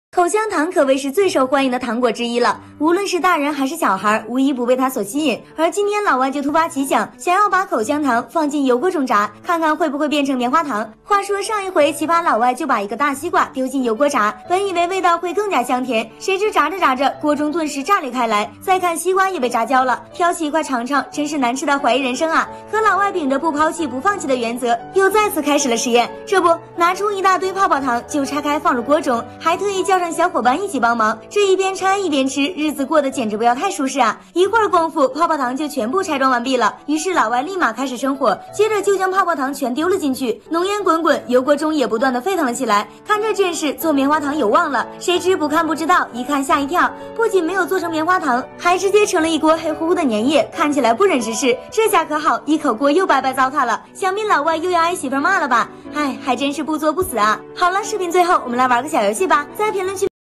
把泡泡糖放进油锅炸会变成棉花糖吗？小伙亲测，结果意想不到
把泡泡糖放进油锅炸会变成棉花糖吗？小伙亲测，结果意想不到,本视频由老彭带你看世界原创提供,34555次播放,好看视频是由百度团队打造的集内涵和颜值于一身的专业短视频聚合平台